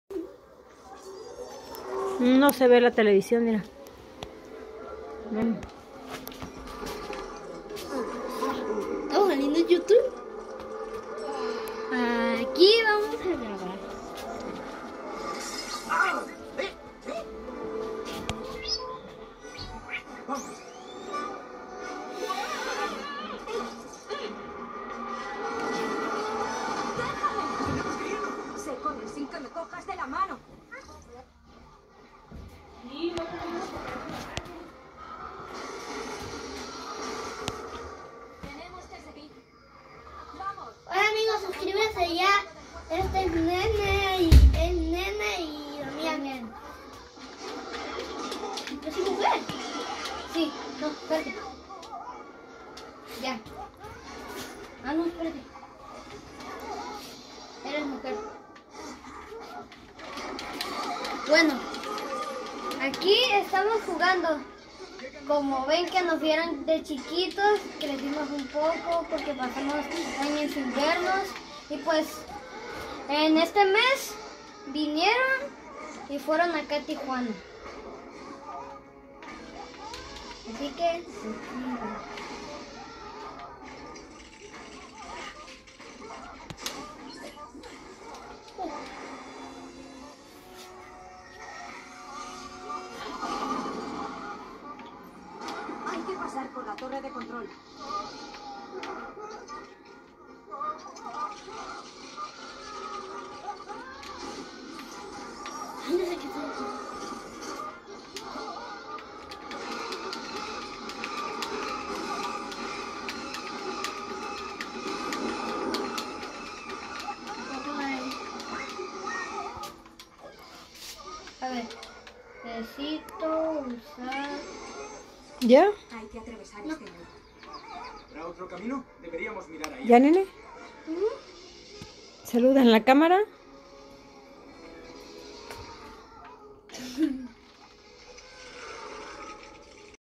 Mis nietos Nene y Damián
De visita en Tijuana por Navidad y fin de año